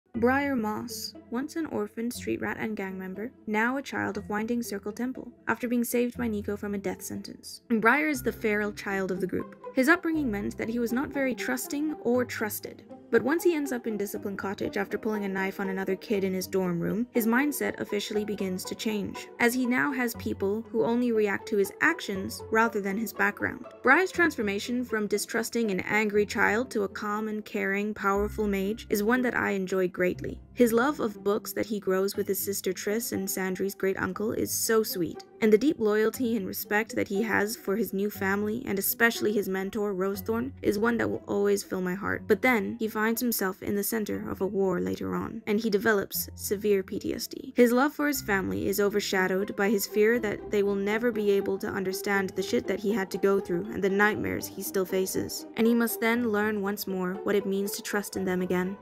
Drawing Winding Circle Characters that I wanna talk about | Part 3 | Briar Moss
Book Series : The Circle of Magic by Tamora Pierce